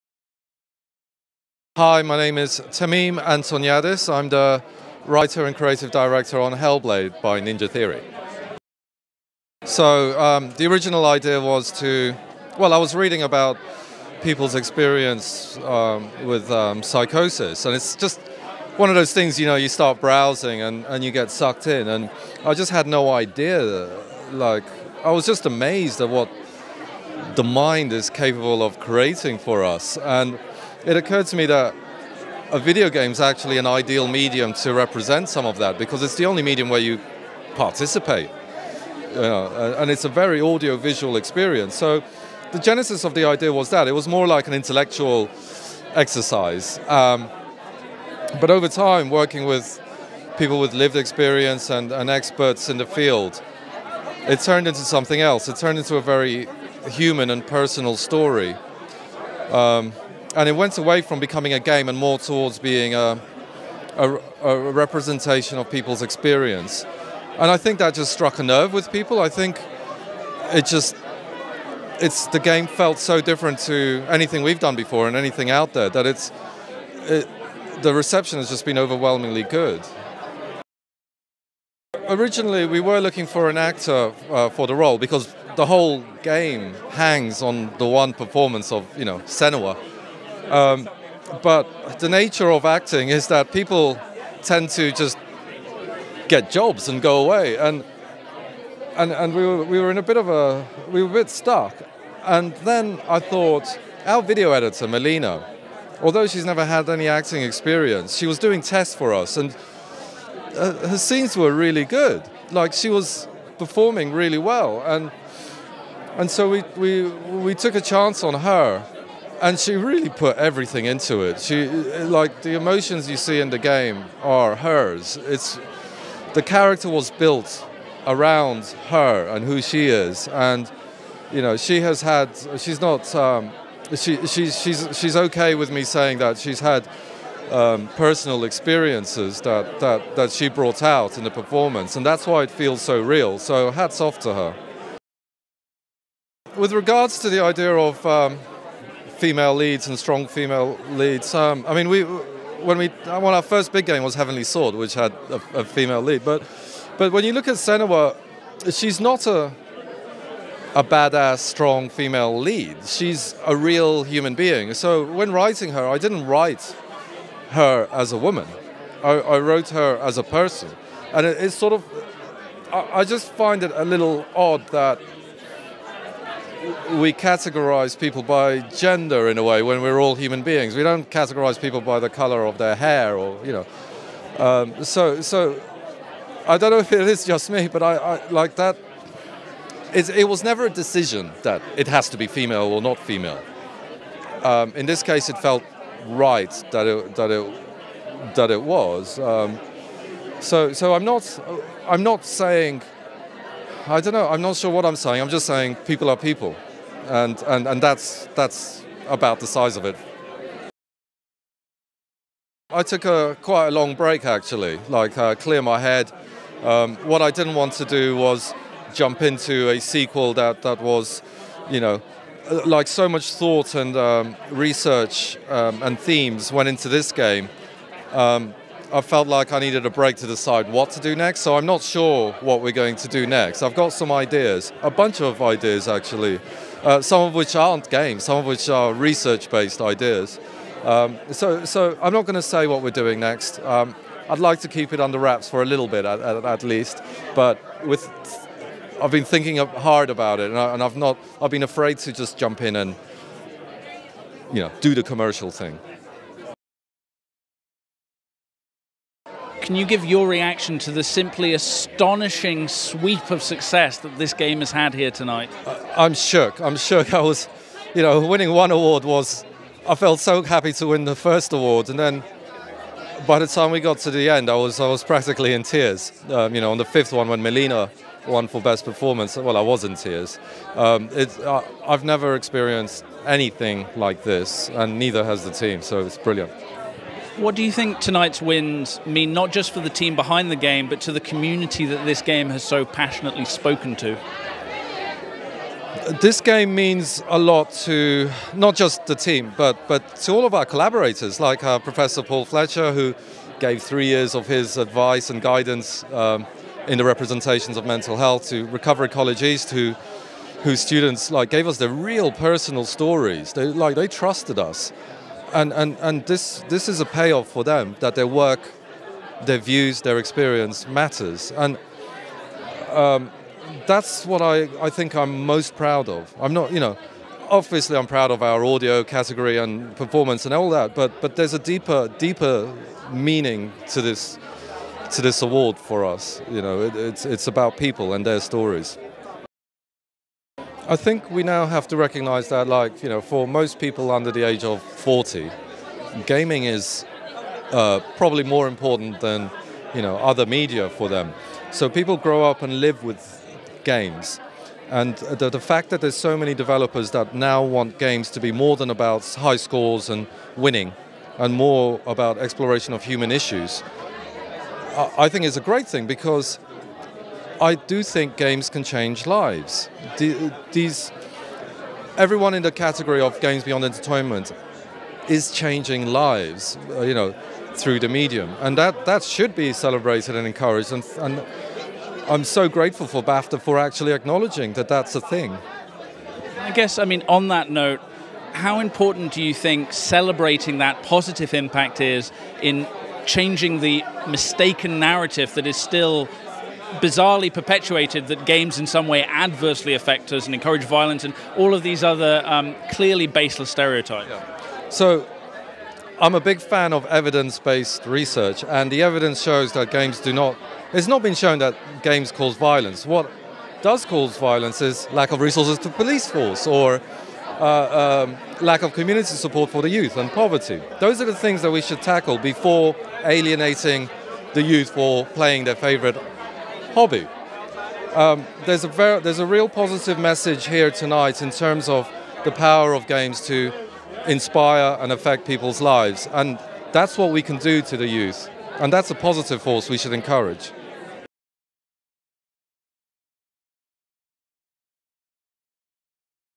Hellblade Senua's Sacrifice Tameen Antoniades Interview
Red Carpet News TV talks to VIP guests, nominees and winners on the red carpet and backstage at the British Academy Games Awards 2018 in London.